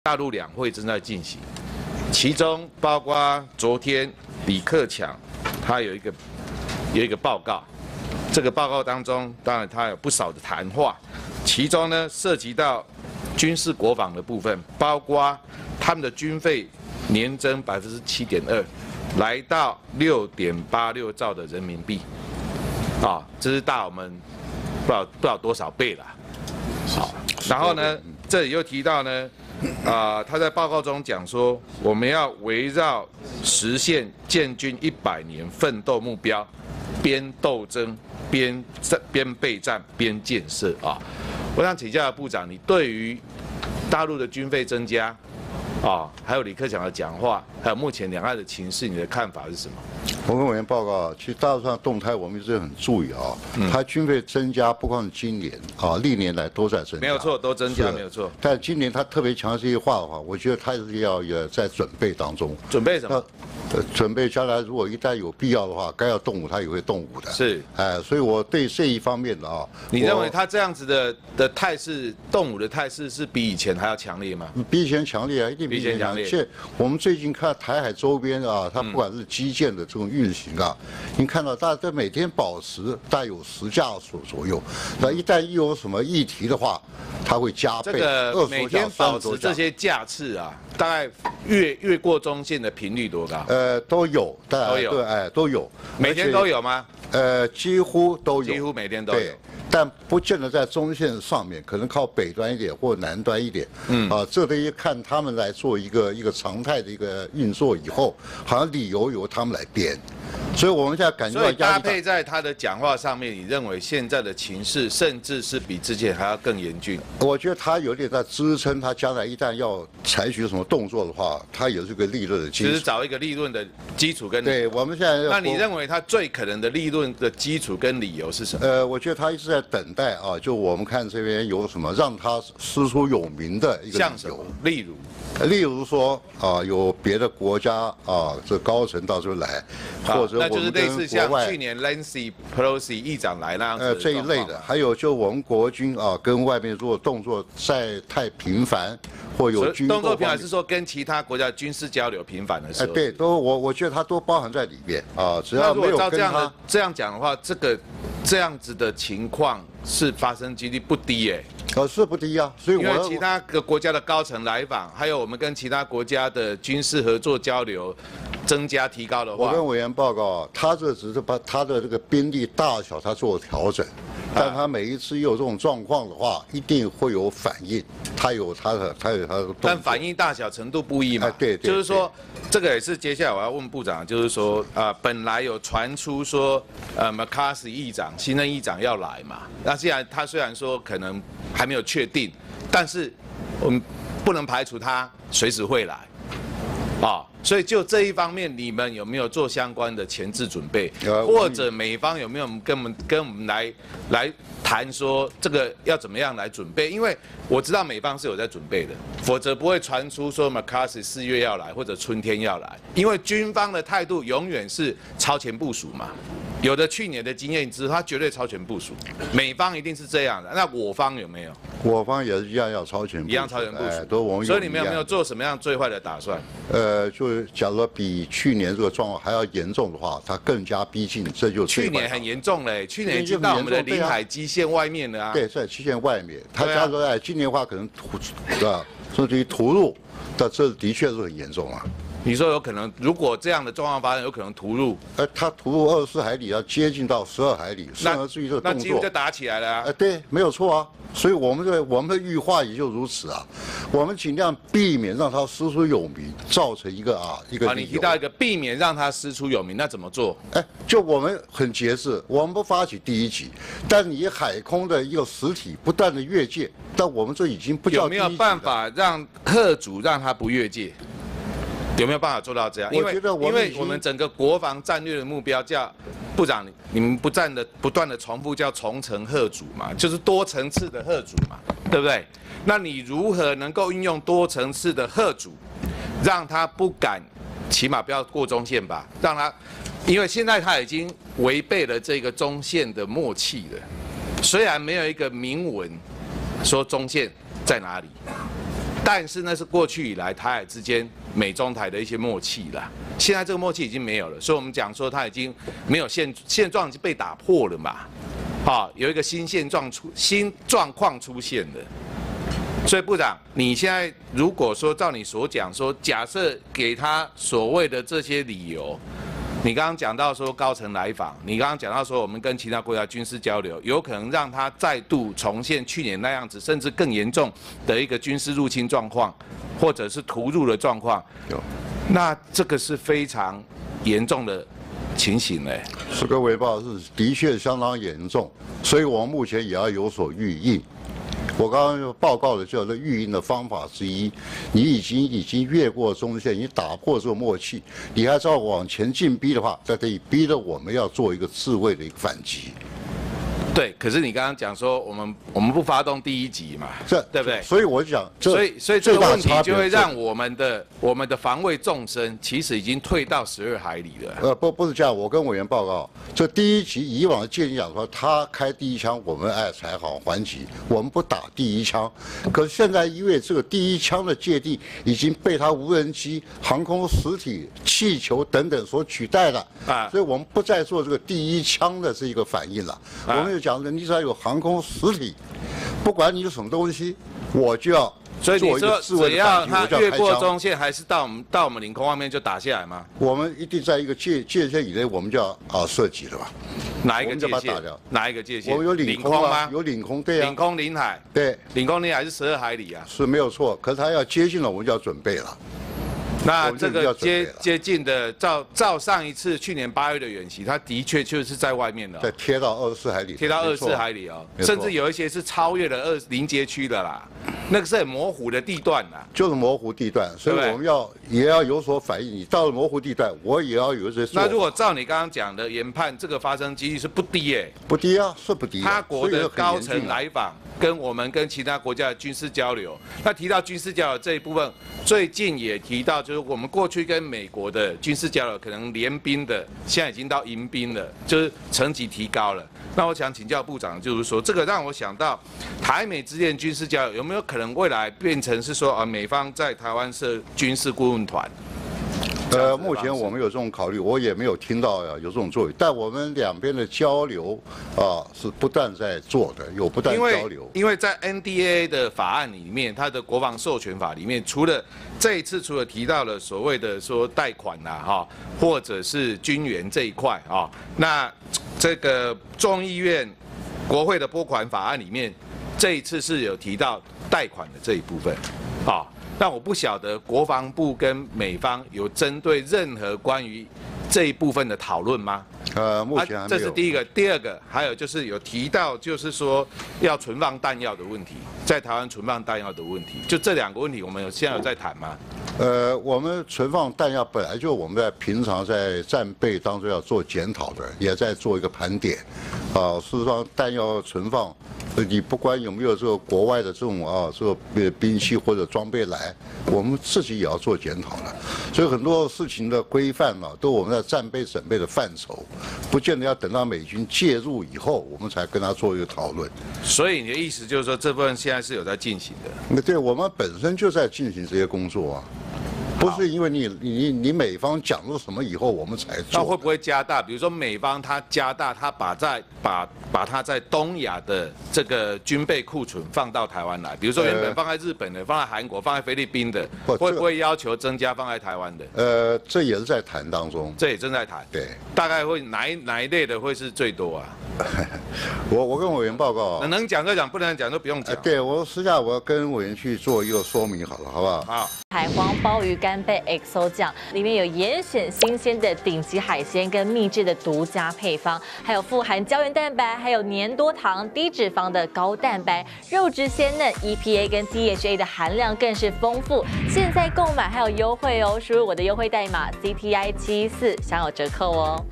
【全程字幕】兩岸兵凶戰危! 傳麥卡錫訪台"共軍恐動武"? 邱國正:若突入12海浬就射擊!?@中天新聞CtiNews
責任小編:快馬加編